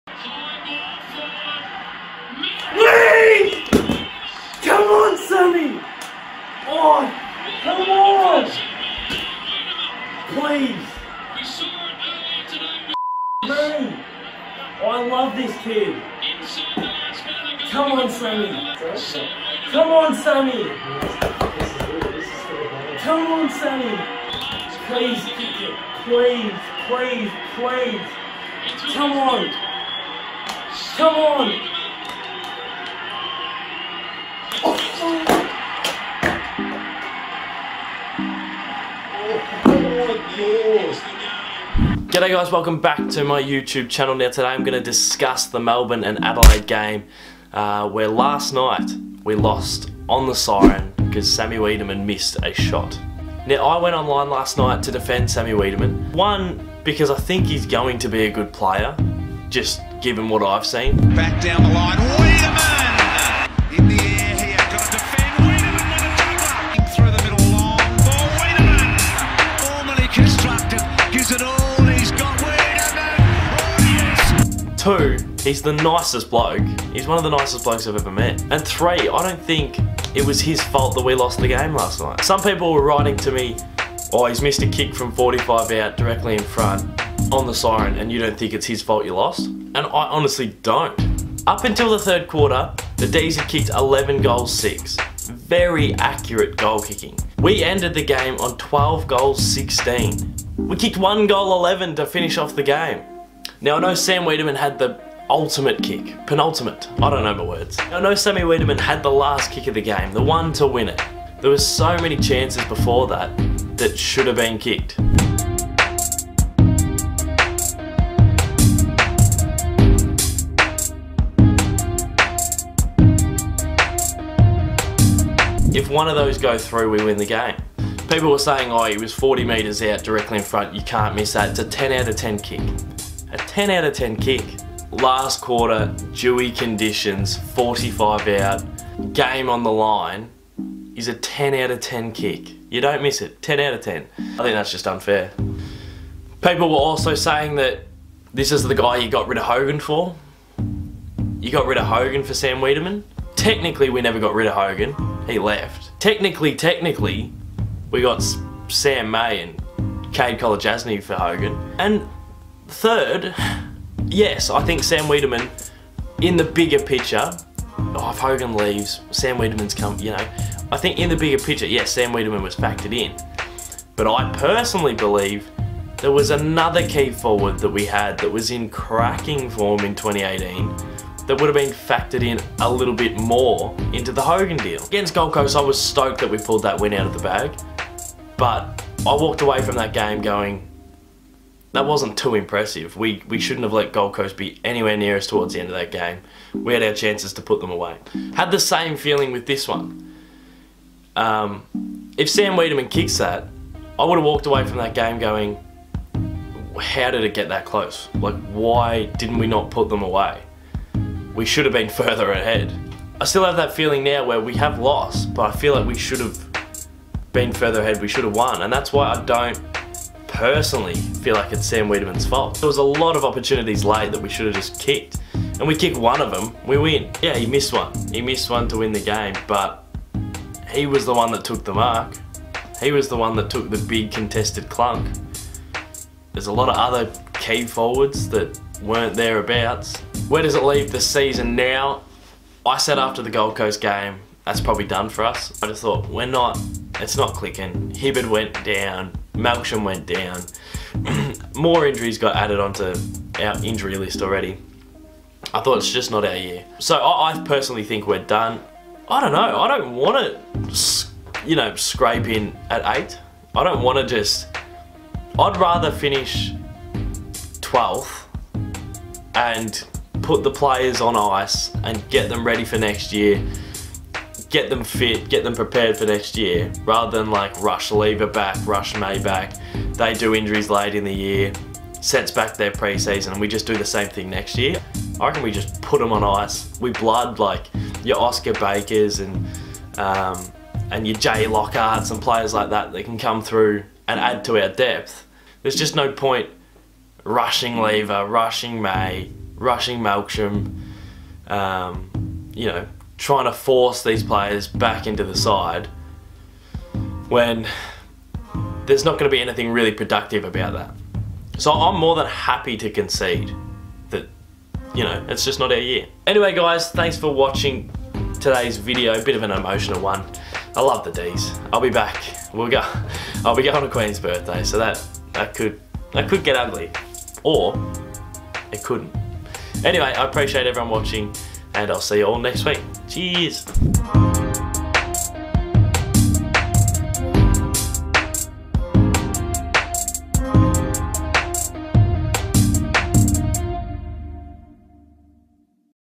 What Happened To My Team? Dees Season Over?
Melbourne Football Club. Shattered about the loss last night. Have been patient for a very long time with this mob, and am prepared to wait a little longer, as frustrating as the loss was, we aren't far off. I can hang tight for one more year.